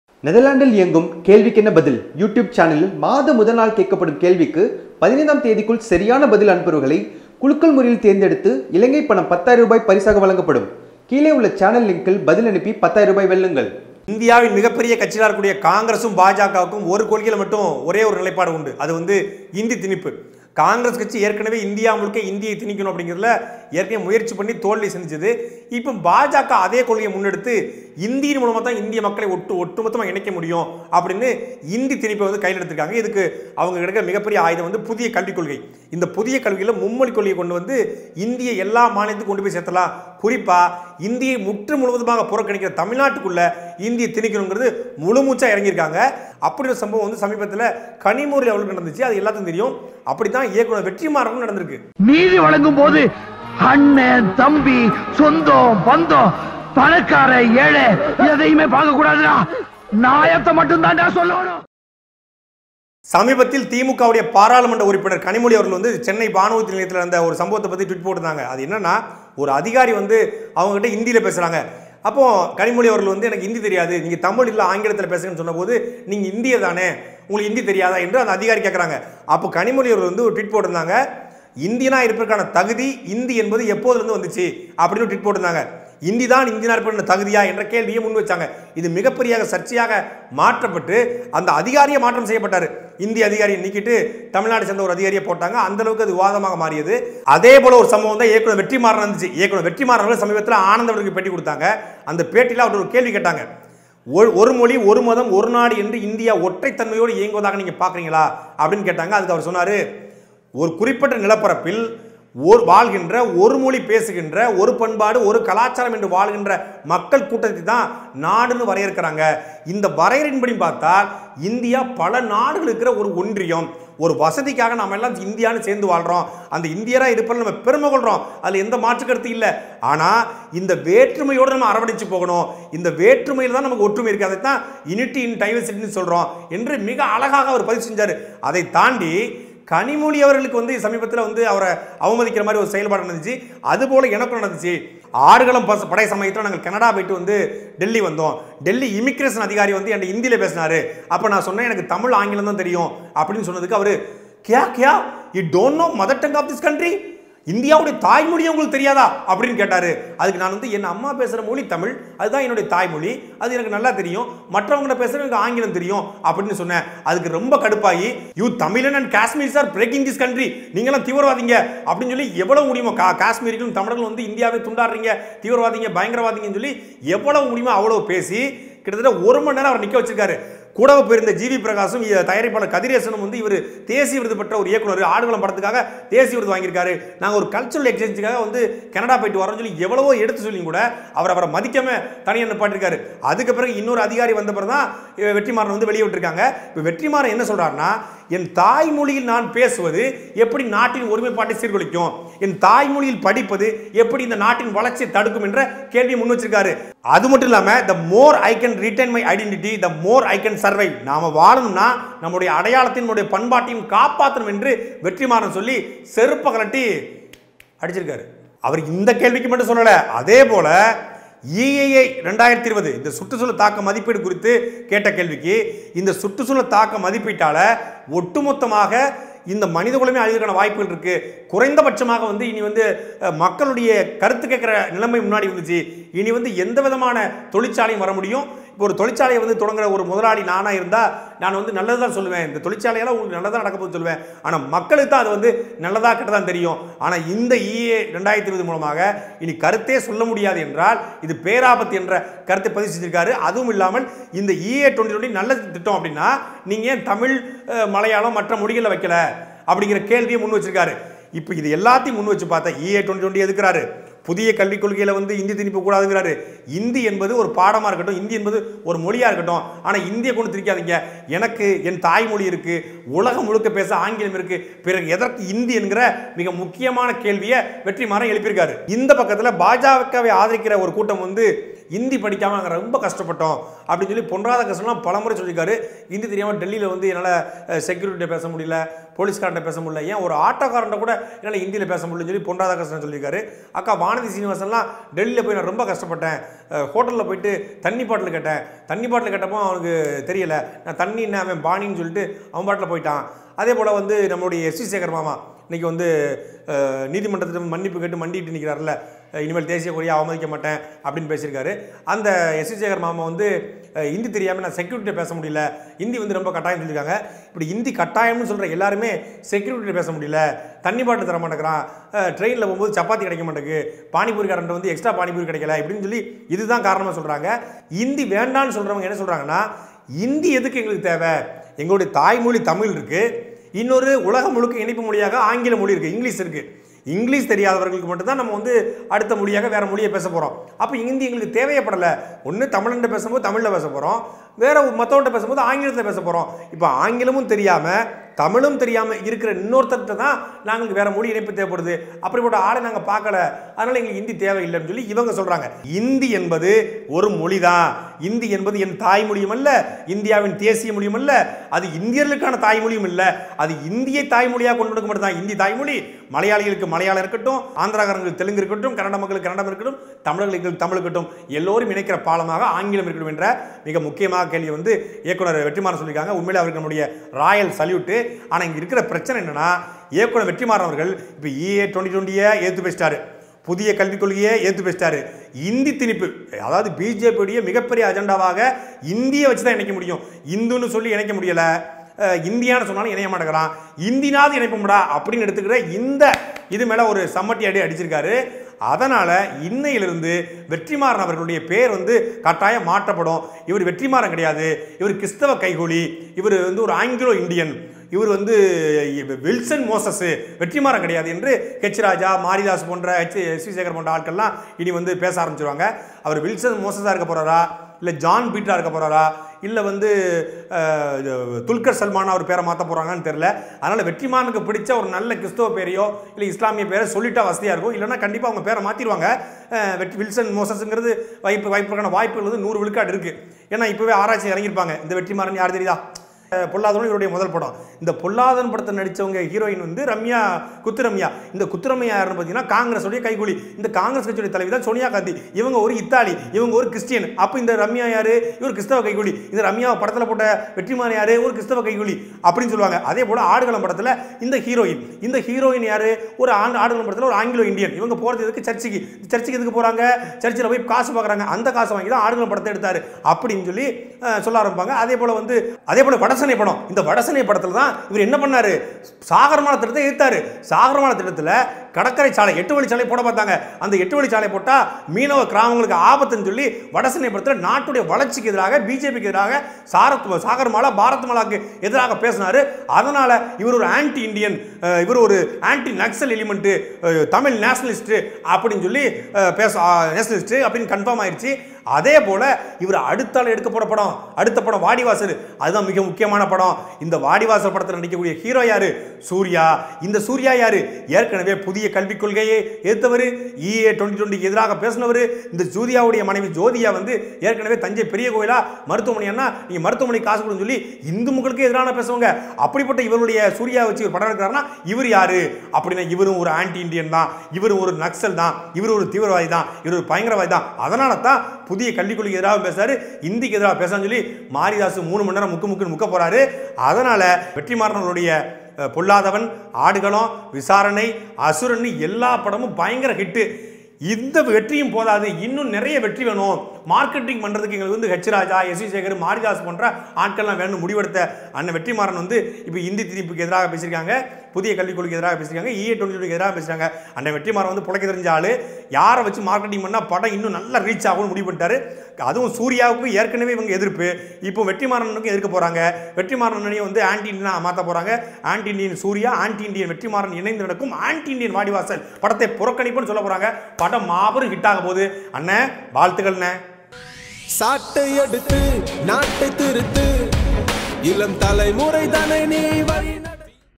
தீவிரவாதி என்று சொல்லி வெற்றிமாறனை ஏர்போட்டில் நிற்க வைத்த அதிகாரி | நாட்டுநடப்பு
புதியகல்விக்கொள்கை மற்றும் சுற்றுச்சூழல் தாக்க மதிப்பீட்டை எதிர்த்து இயக்குனர் வெற்றிமாறன் பேசியதால் இனி அவர் அன்ட்டி இண்டியன் என்று அழைக்கப்படுவார்

கேள்விக்கு என்ன பதில் சேனலில் வரும் கேள்விக்கு பதில் சொல்லி 10 ஆயிரம் வெல்லுங்கள்